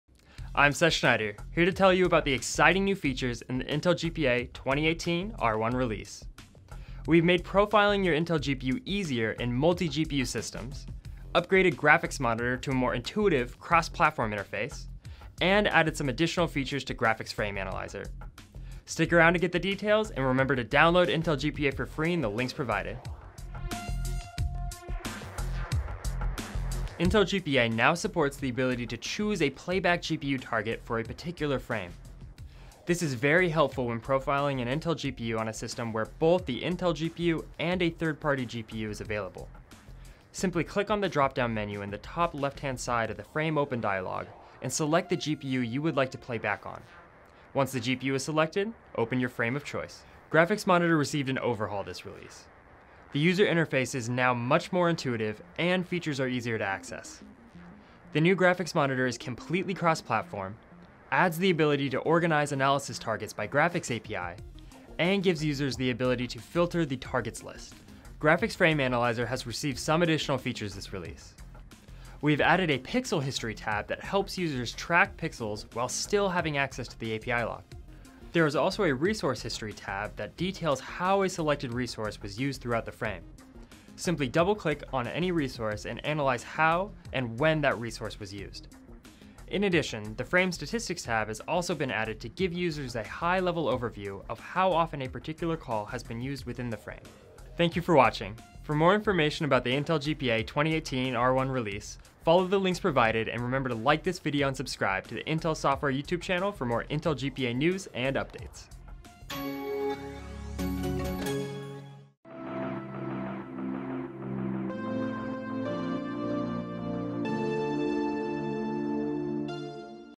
Intel® Graphics Performance Analyzers 2018 R1 Release | Intel Software
Intel® GPA provides tools for graphics analysis and optimization that can help you make games and other graphics-intensive applications run even faster. The tools support the platforms based on the latest generations of Intel Core™ and Intel Atom™ processor families, for applications developed for Windows, Android, or Ubuntu* OS.

In this release, we have made profiling your Intel GPU easier in multi-GPU systems by adding the ability to choose a GPU playback target, upgraded Graphics Monitor to a more intuitive cross platform interface, and added some additional features to Graphics Frame Analyzer.